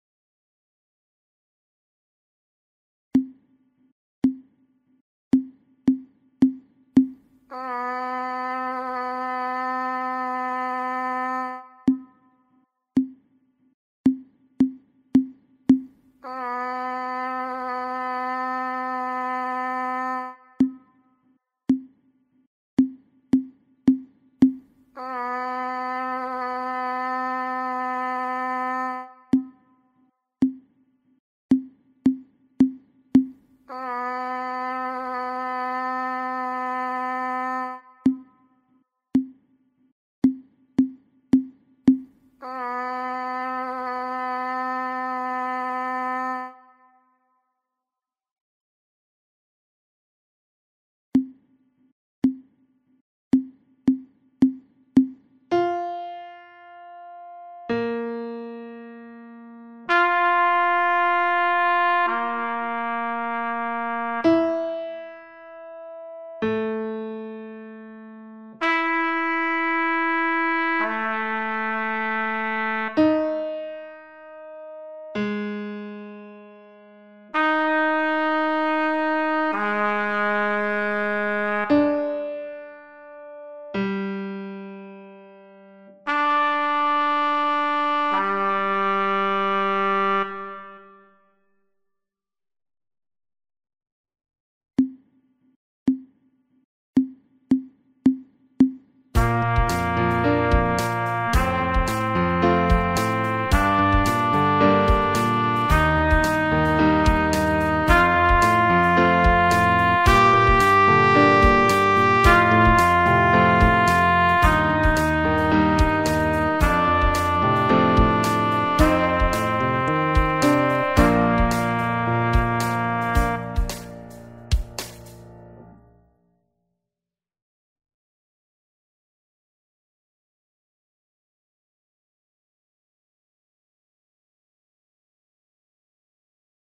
Warm Up 5 - Trumpet Solo with Warm Up and Play Along
This is a Trumpet Warm up for beginner players. It features three useful activities all of which uses only six notes.

Many thanks